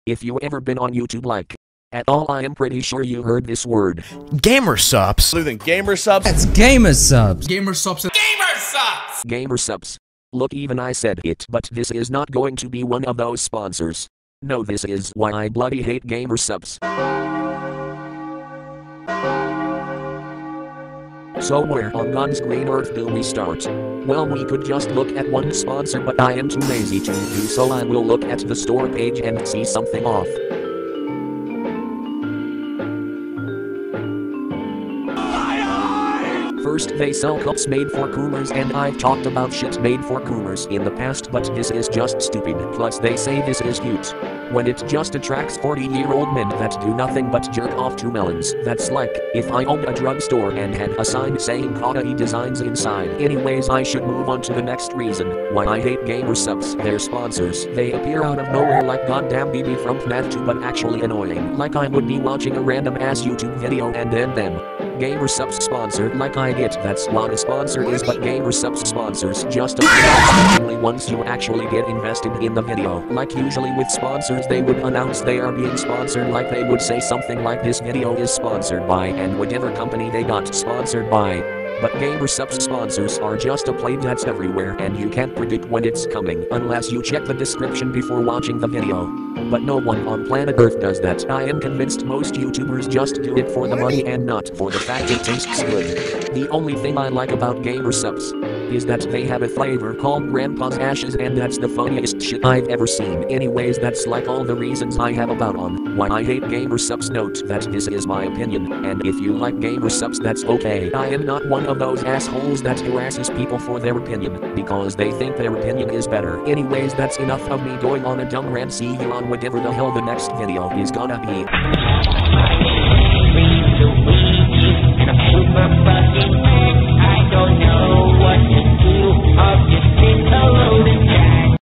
Why i HATE Gamersupps
again this is my opinion and if you like gamersupps that totally fine just dont be a prick and harass me for hating something you like
(socials)